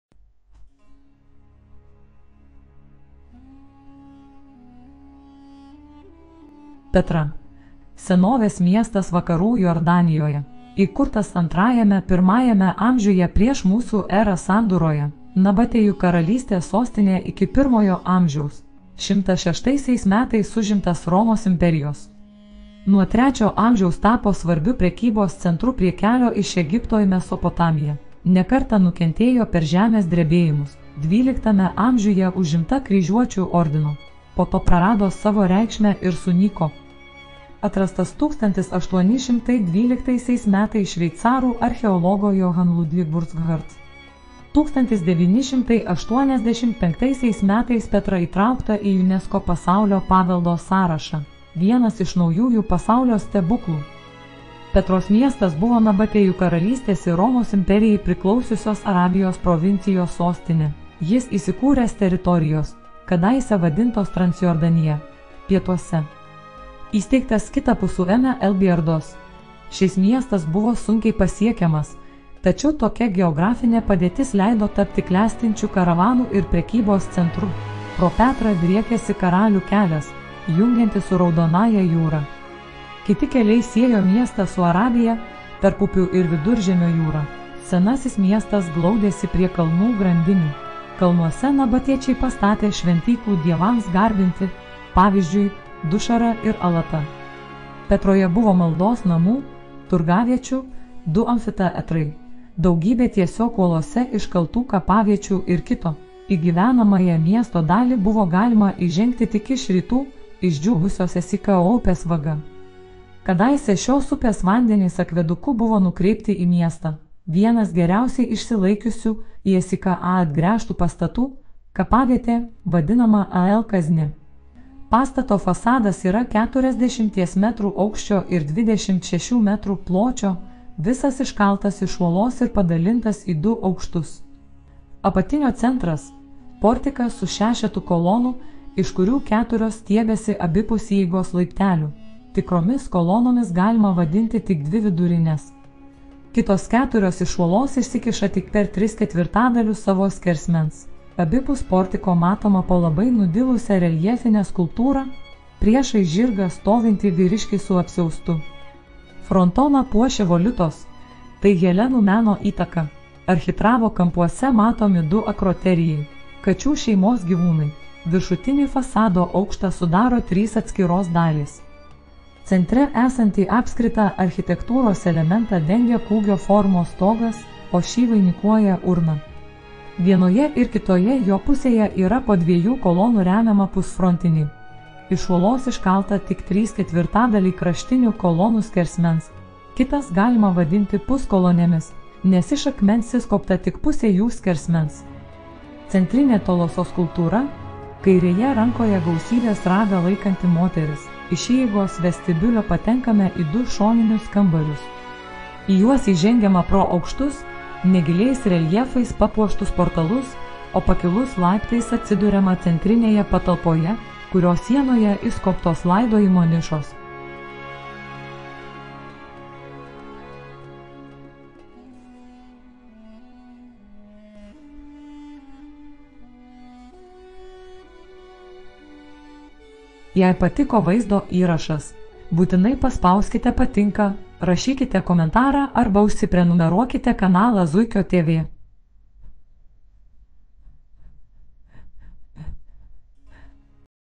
Petra – senovės miestas Vakarų Jordanijoje
Petra – senovės miestas Vakarų Jordanijoje. Įkurtas II–I amžiuje prieš mūsų erą. sandūroje. Nabatėjų karalystės sostinė iki pirmojo amžiaus. 106 metais užimtas Romos imperijos. Nuo III amžiaus tapo svarbiu prekybos centru prie kelio iš Egipto į Mesopotamiją. Ne kartą nukentėjo per žemės drebėjimus. XII amžiuje užimta kryžiuočių ordino. Po to prarado savo reikšmę ir sunyko. Atrastas 1812 metais šveicarų archeologo Johann Ludwig Burckhardt. 1985 metais Petra įtraukta į UNESCO Pasaulio paveldo sąrašą.